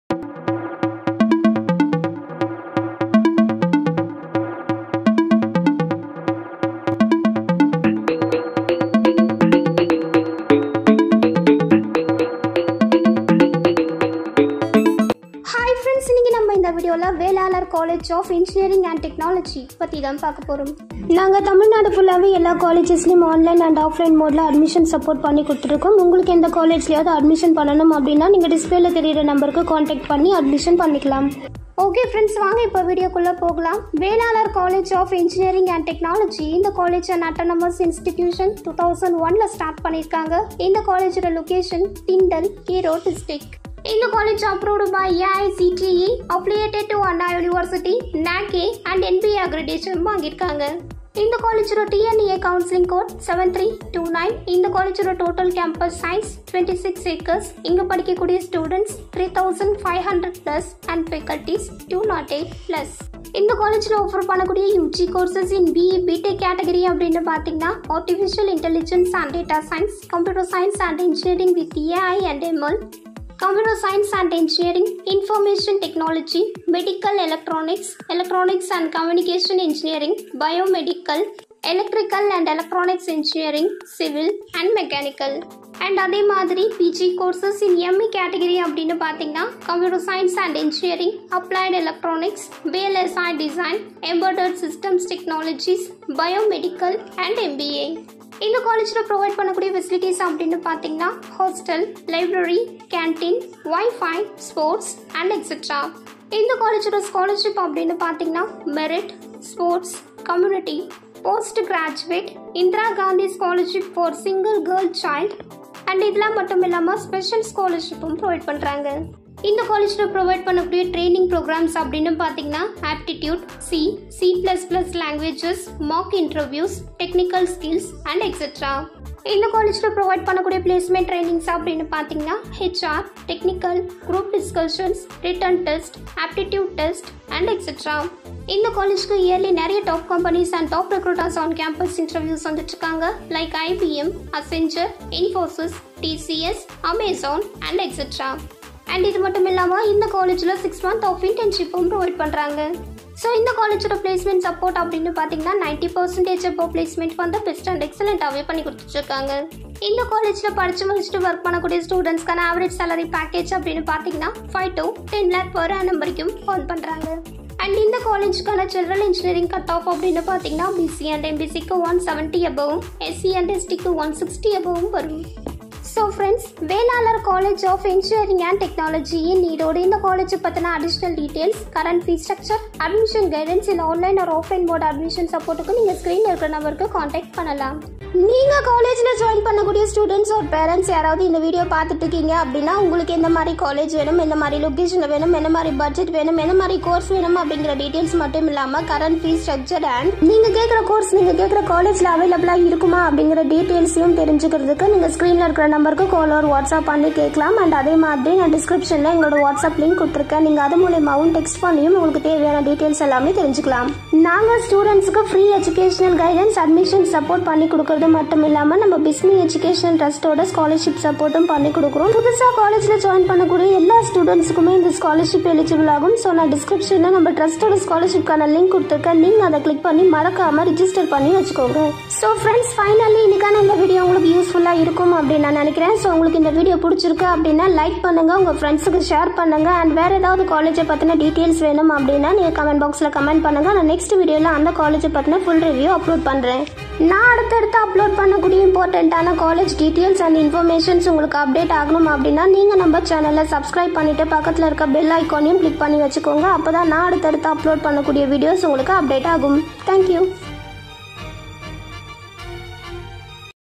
VELALAR COLLEGE OF ENGINEERING & TECHNOLOGY AT ERODE DISTRICT. COURSES / PLACEMENT / SALARY / CUTOFF
Video Included :

Scope of Information Technology, ADMISSION HELPLINE : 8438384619 / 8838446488

Over view : About Course, Eligibility, Entrance Exams, Career Opportunities, Job Role, Top Recruiters, Salary Package Details.

Cheers!